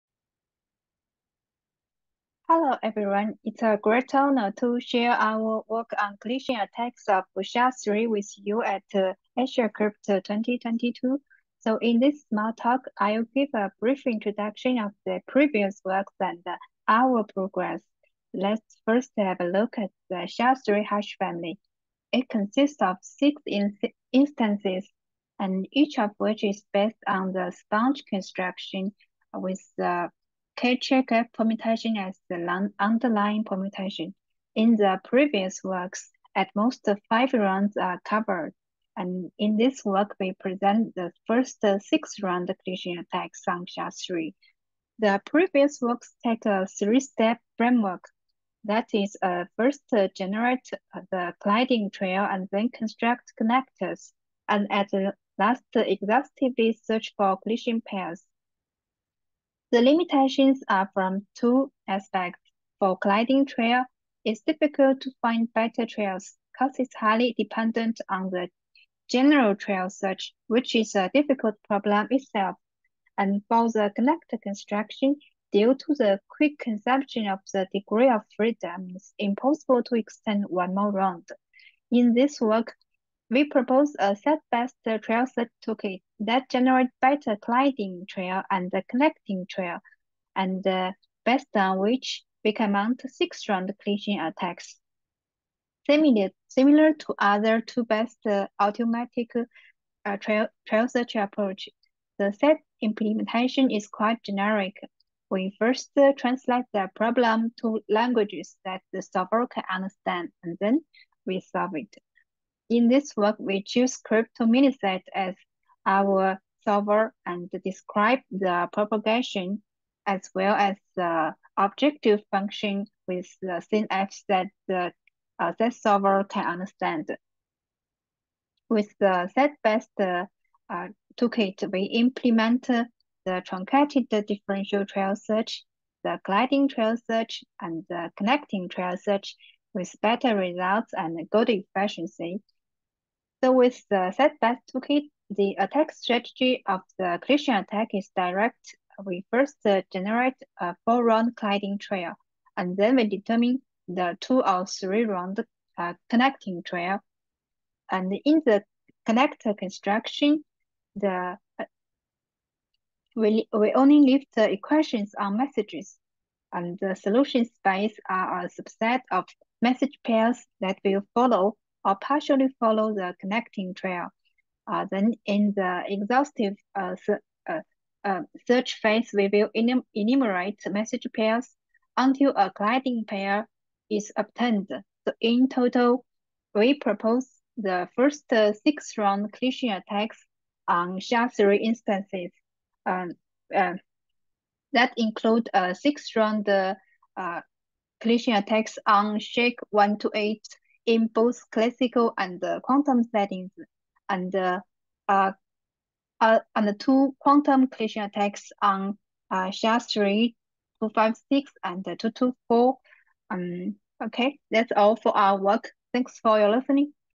Exploring SAT for Cryptanalysis: (Quantum) Collision Attacks against 6-Round SHA-3
Paper by Jian Guo, Guozhen Liu, Ling Song, Yi Tu presented at Asiacrypt 2022